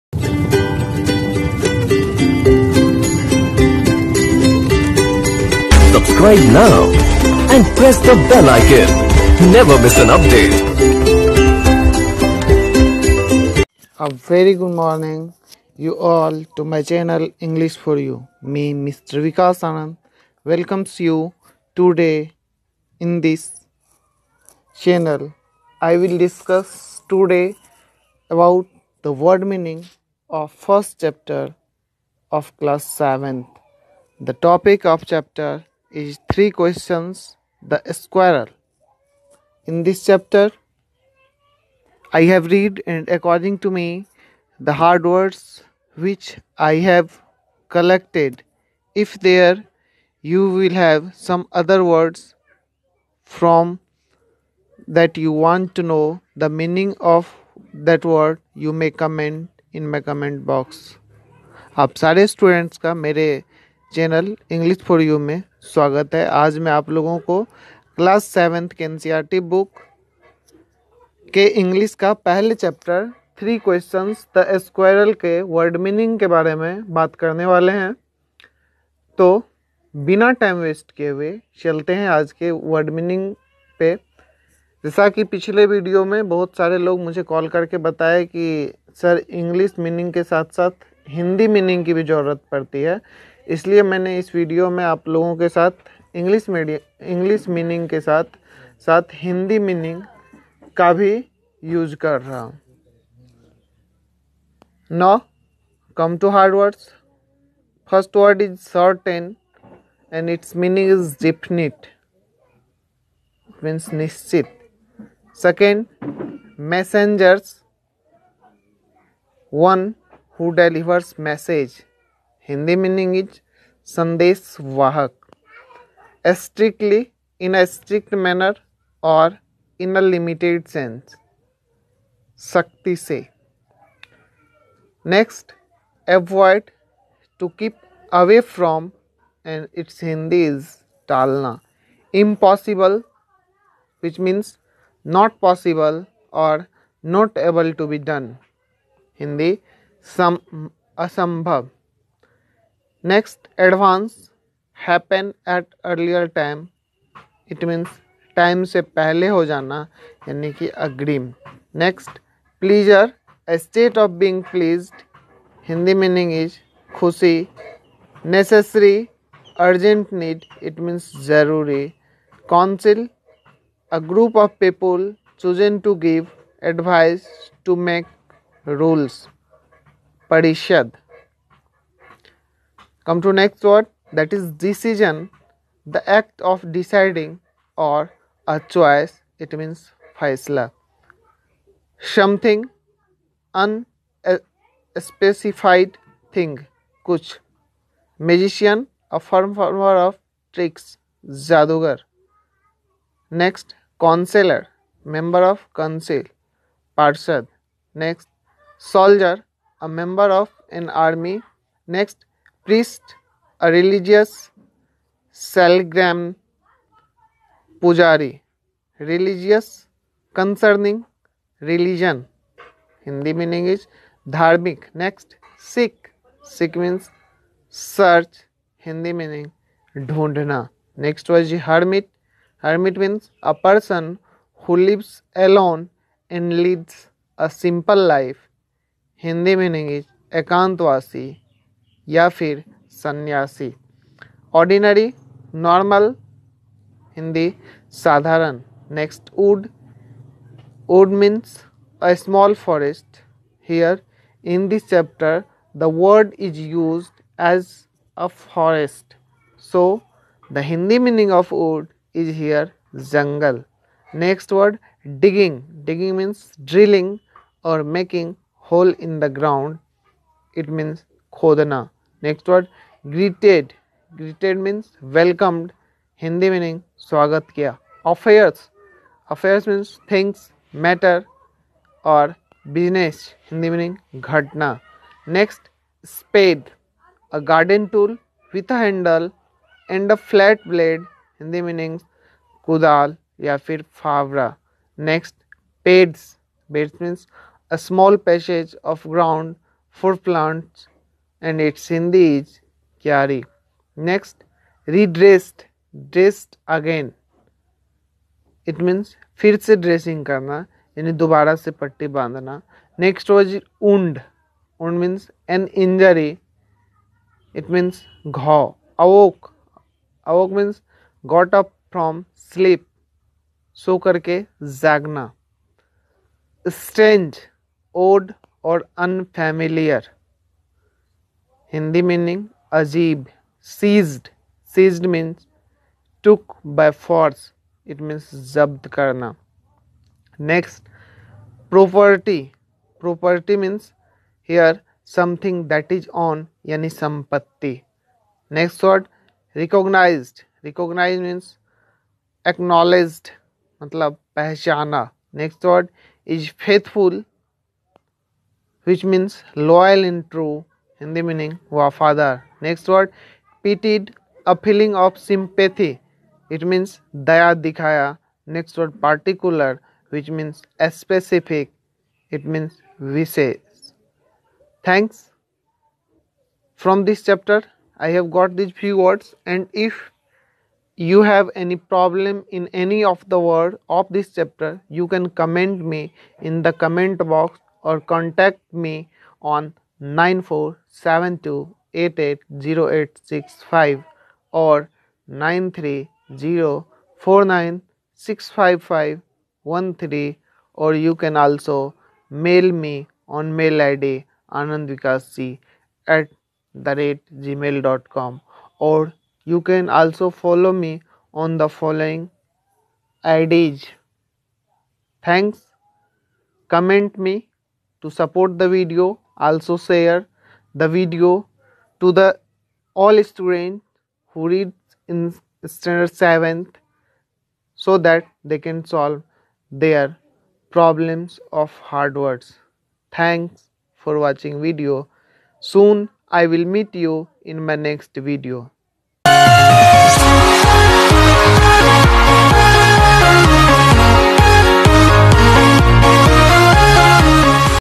NCERT ENGLISH HONECOMB CLASS -7 (LESSON-1) WORDMEANING|| By
The wordmeaning of whole chapter is explained thoroughly.